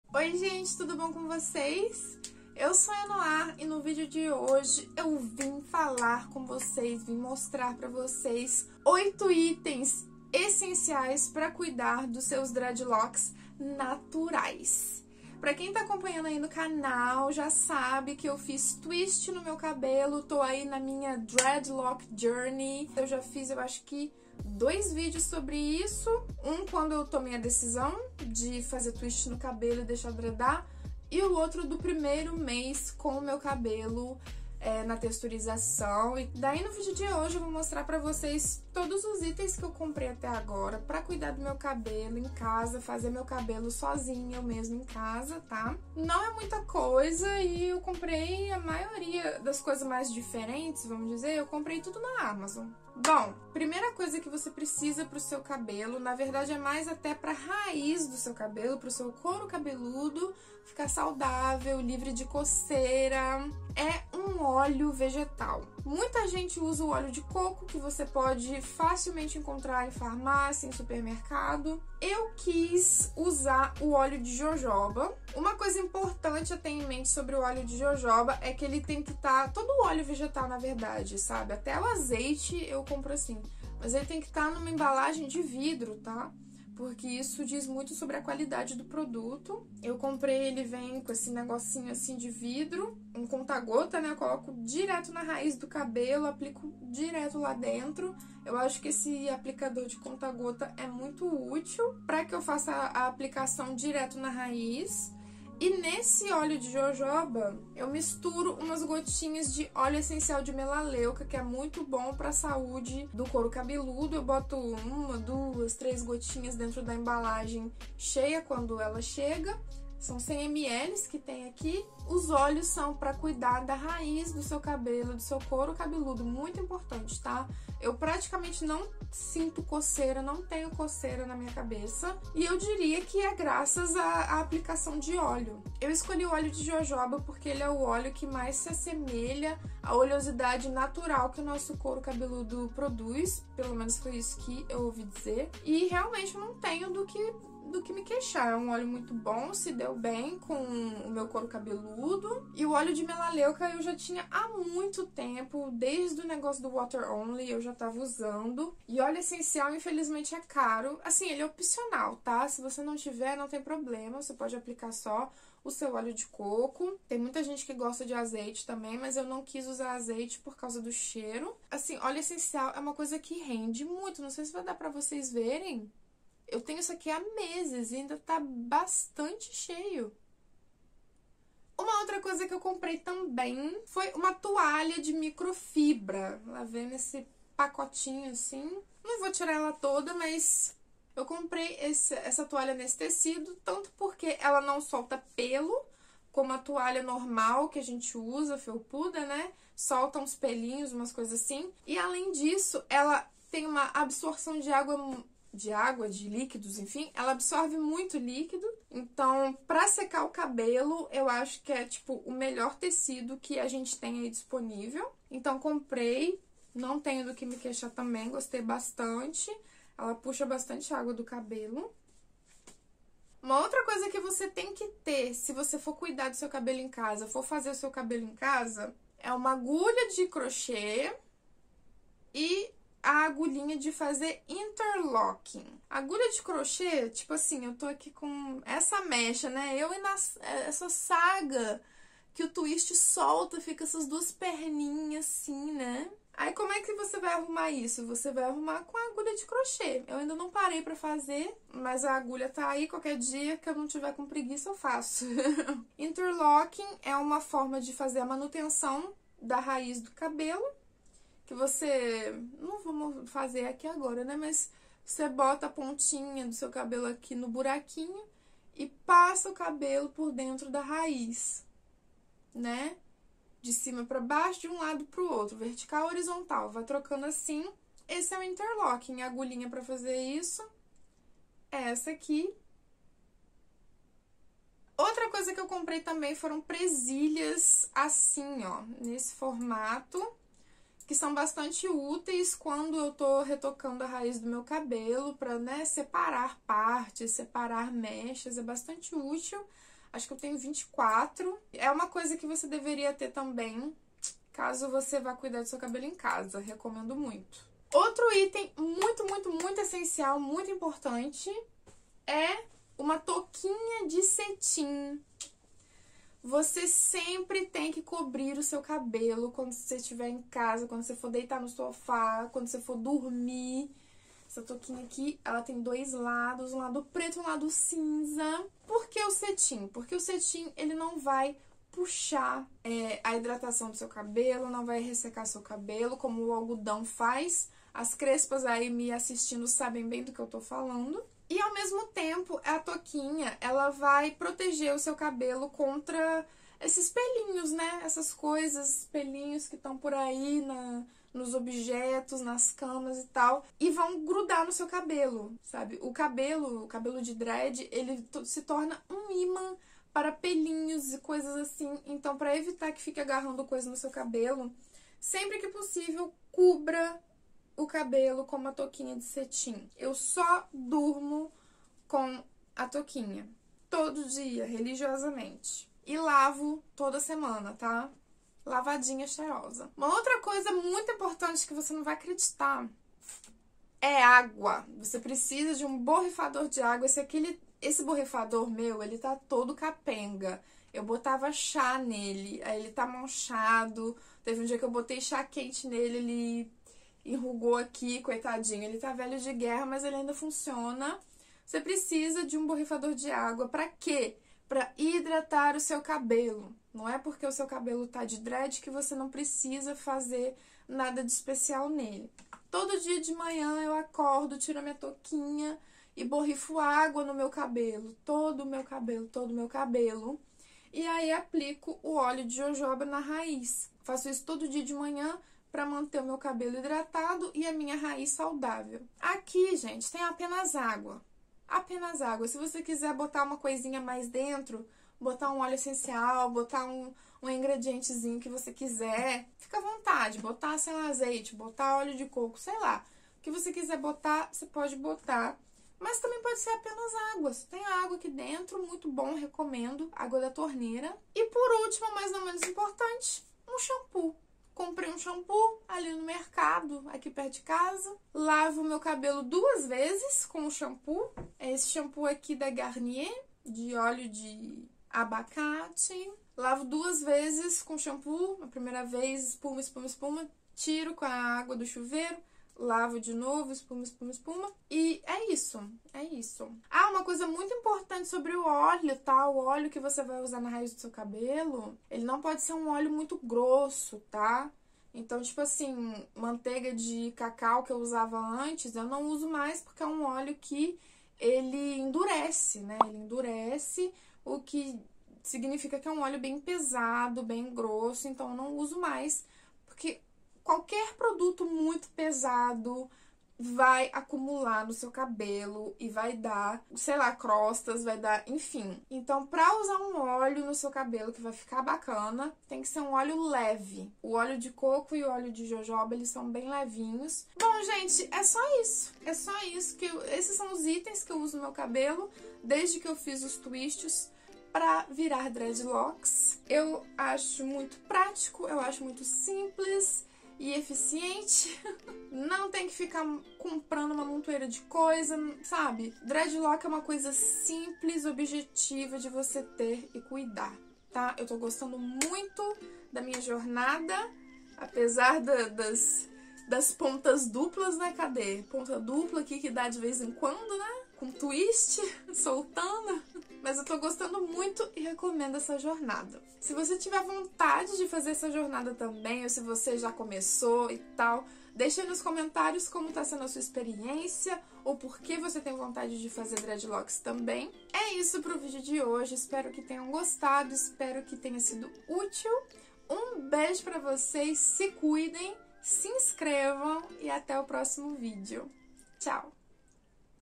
Itens essenciais para cuidar de dreadlocks naturais
Vc tem dreads naturais e tá procurando saber de coisas que vc precisa pra cuidar deles? Neste vídeo eu mostro pra vcs 8 itens essencias pra vc cuidar dos seus dreadlocks! São coisas bem simples, mas que fazem toda a diferença para a saúde dos dreadlocks e do couro cabeludo. Espero que gostem! :)

Óleo de Jojoba
Óleo de côco
Óleo essencial de melaleuca
Toalha de microfibra
Conjunto de agulhas para dreadlocks
Presilhas de cabelo
Touca de cetim
Borrifador de água
Shampoo antiresíduos vegano

🌻 O que tem neste canal?
Estilo de vida, uns livros e uns surtos, ~quase todo domingo ao meio dia.